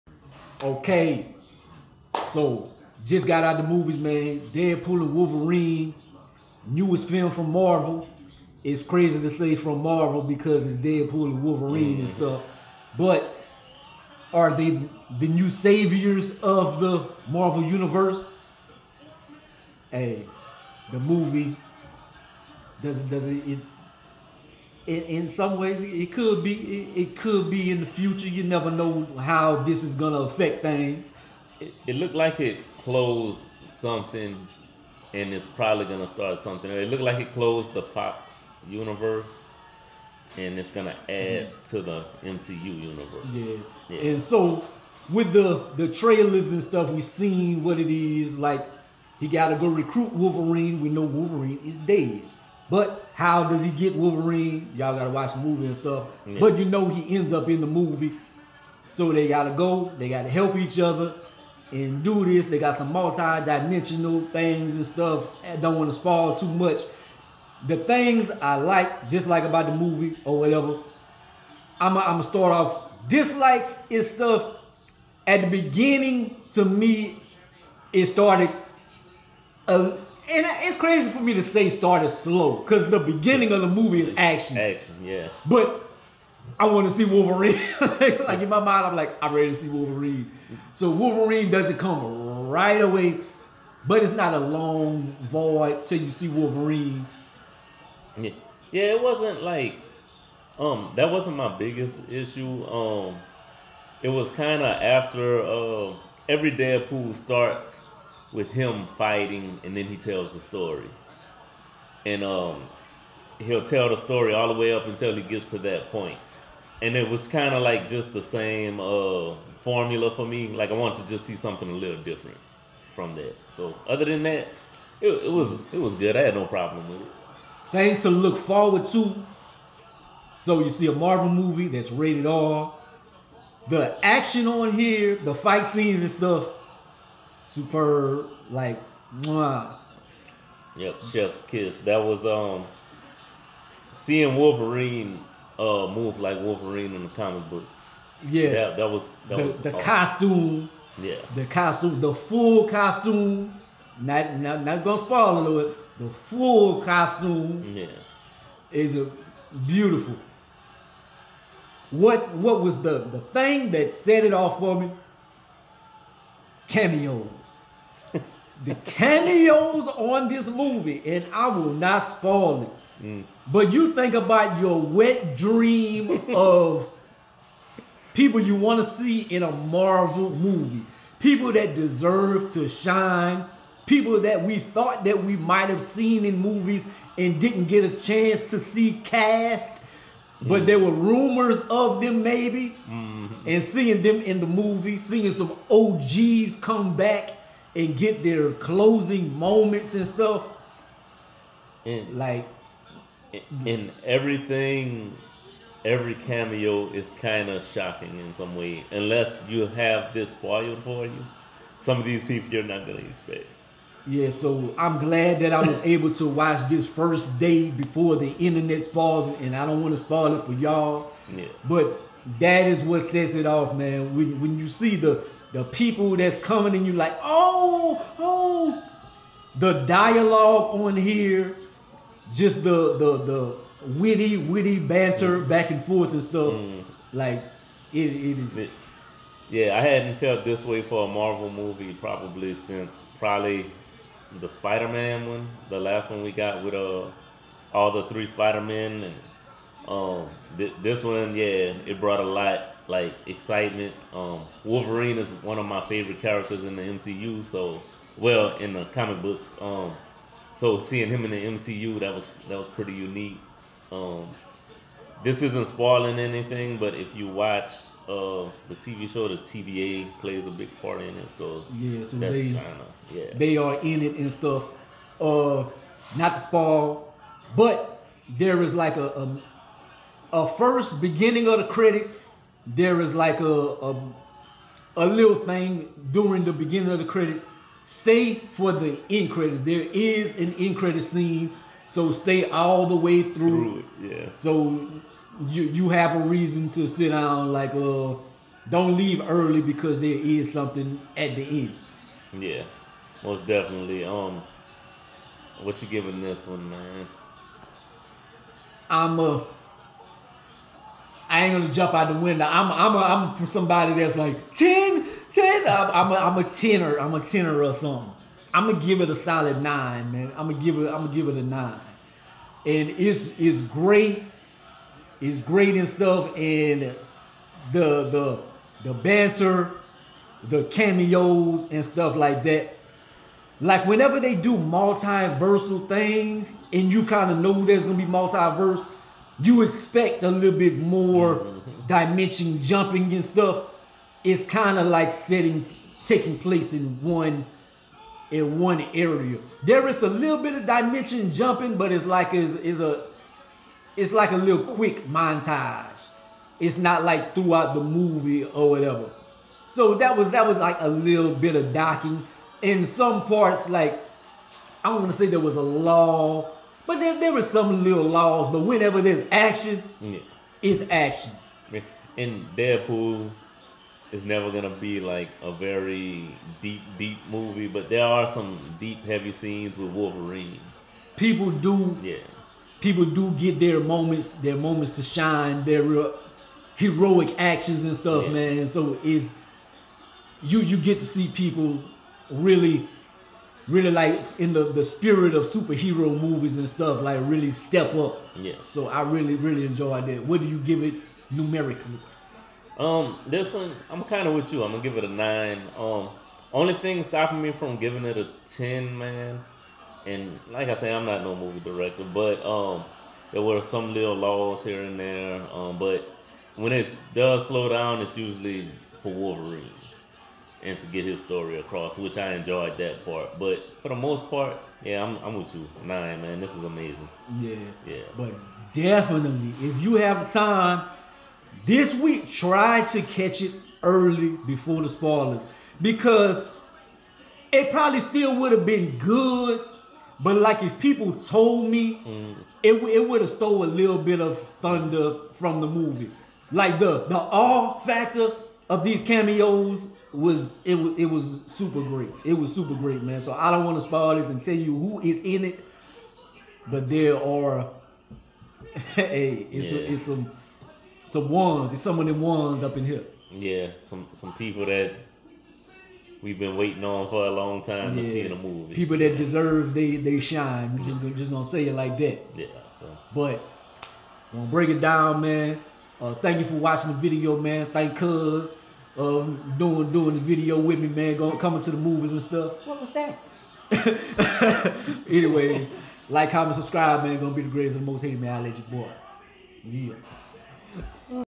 DEADPOOL & WOLVERINE Movie Review | Why U Must See It!
Wolverine is recovering from his injuries when he crosses paths with the loudmouth, Deadpool. They team up to defeat a common enemy.